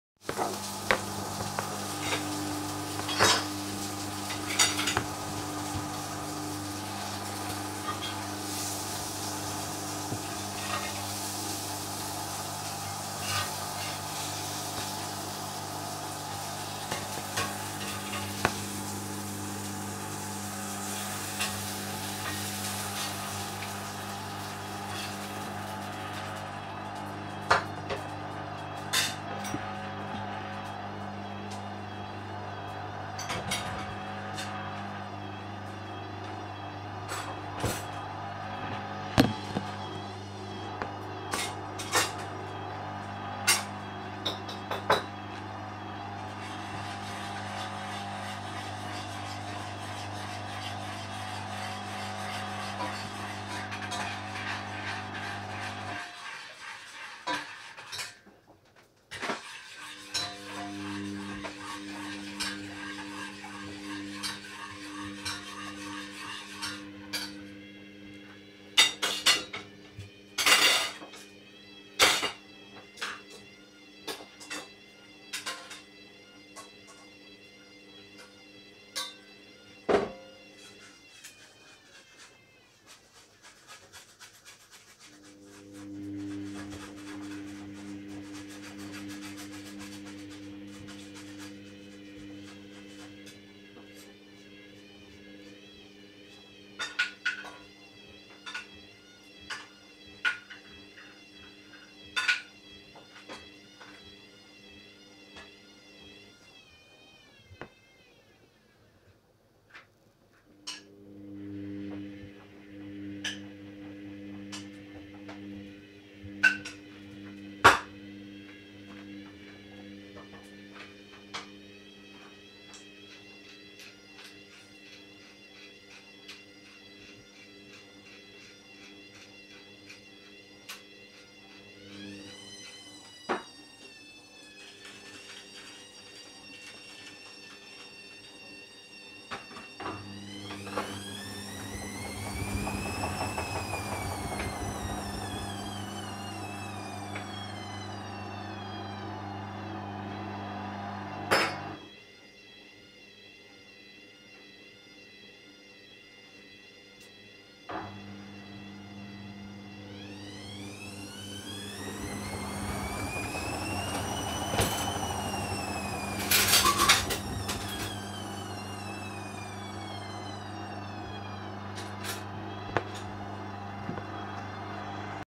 Hoover dynamic coloureds 40 c sensitive care part 8
We are on fourth spin very unbalanced with a sudslock.